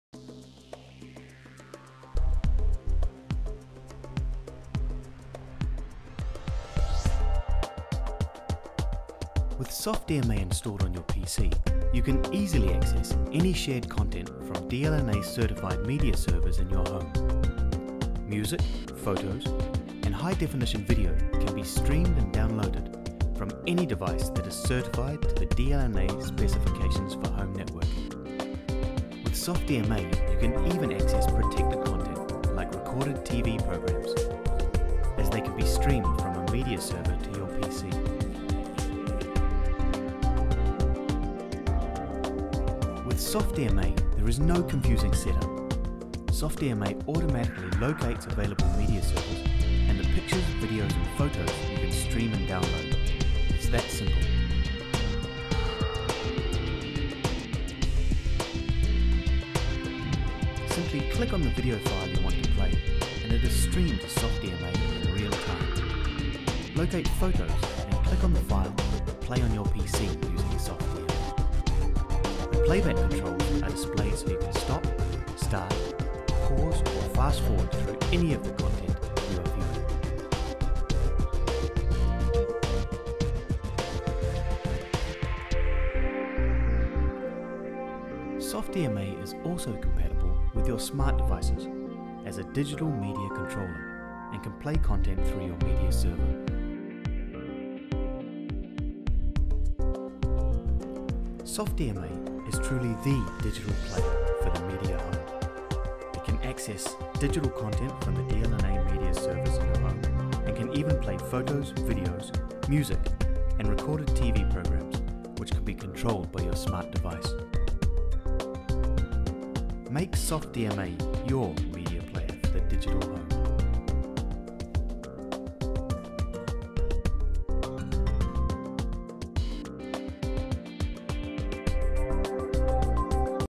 SoftDMA 2 製品紹介ビデオ (日本語字幕つき)
DTCP-IPに対応したデジタルメディアプレイヤーSoftDMA 2の製品紹介ビデオです。SoftDMA 2はDTCP-IPに対応しているので、録画した地デジ番組をPCでストリーミングによる視聴が行えます。